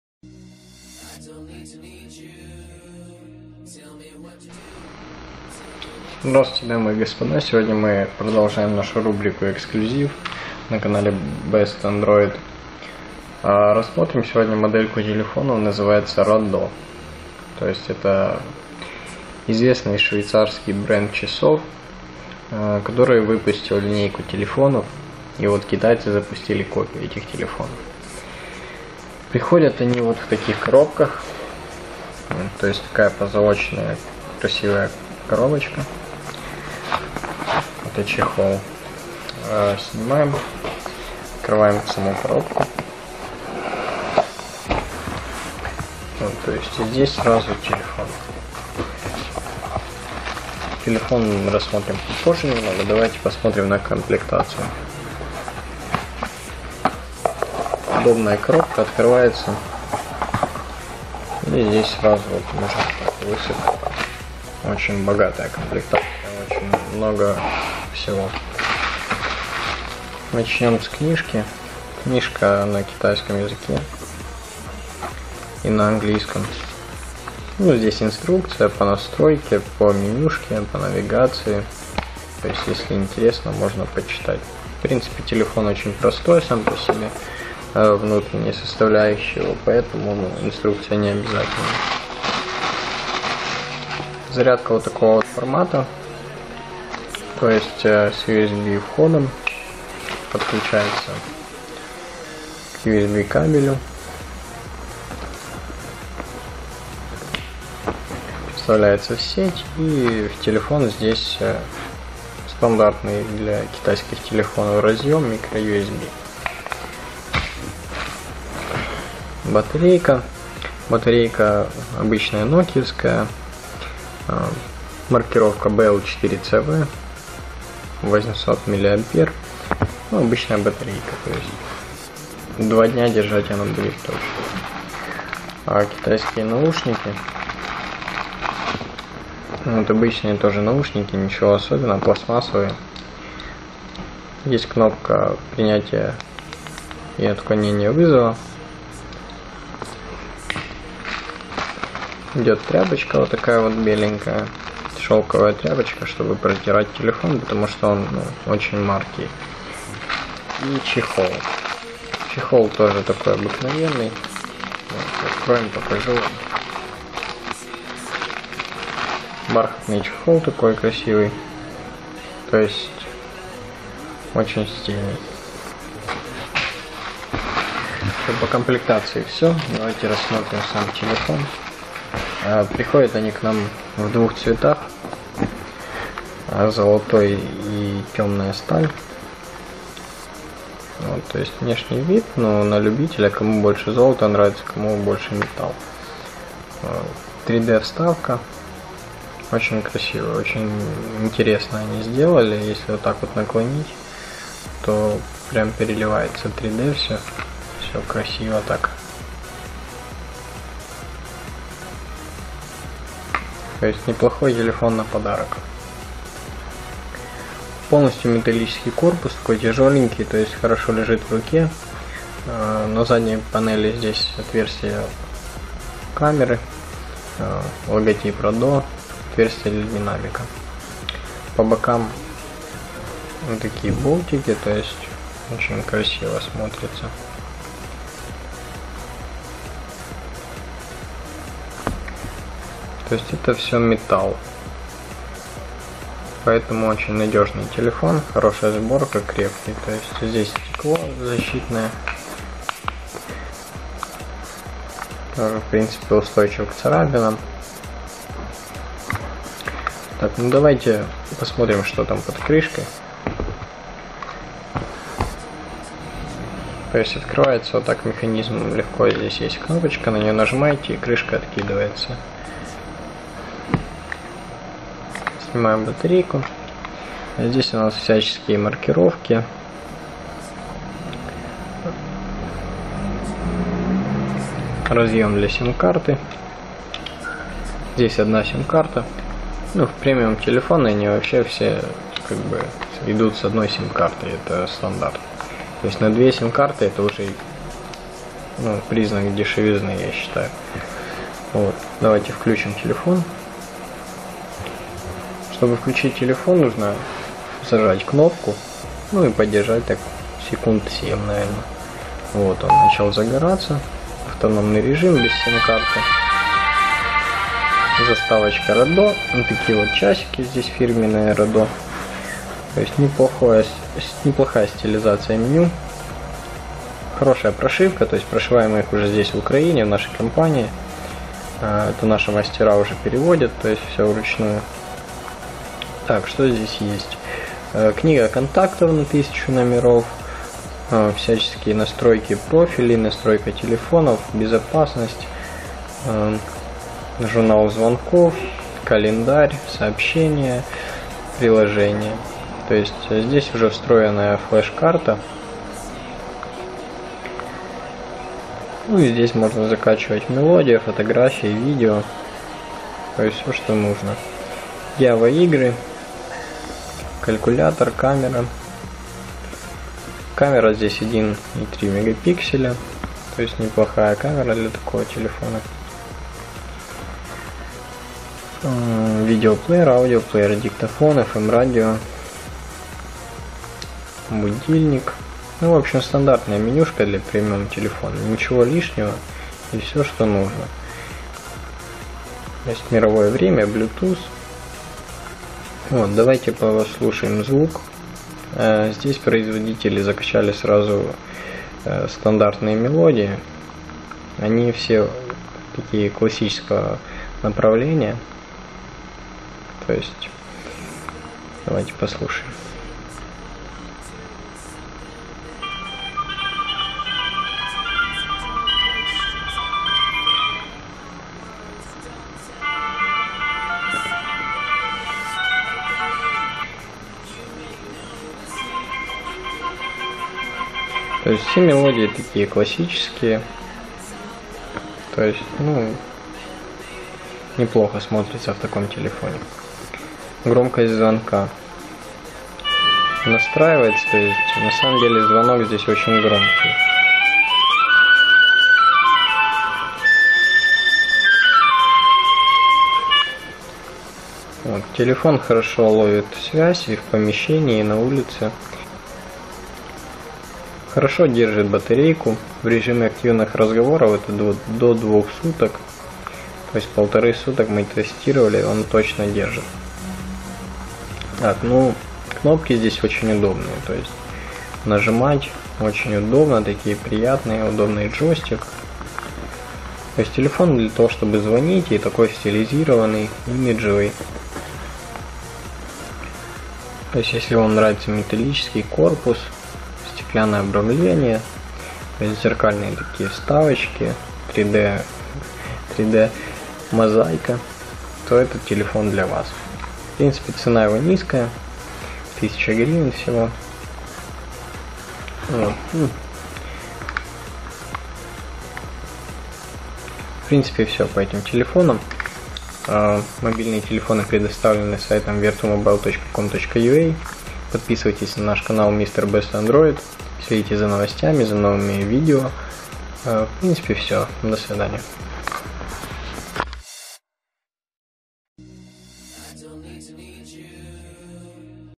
Телефон_Rado_R800_Распаковка. Элитный_Телефон_Rado
Вы знакомы с часами компании Радо, но еще не знаете мобильные телефоны этой компании? Ваша философия –
быть не похожими на других? Именно эту цель преследовала компания Rado, выпуская свои уникальные телефоны,
и Вы вполне можете стать обладателем недорогой копии телефона Rado R800, которую вряд ли можно найти у
Ваших друзей и знакомых. Уникальная копия Радо R800 – это цельный металлический корпус, оригинальный
дизайн, продуманная конструкция клавиш и джойстика, неповторимый зеркальный дисплей – одним словом,
уникальный, неповторимый аксессуар бизнес класса!
Китайская копия телефона RADO. Богатая комплектация, Интересной формы золотой или черный с 3 D вставкой
в металлическом корпусе. Очень красивый телефон в шикарной золоченой упаковке, подойдет на подарок.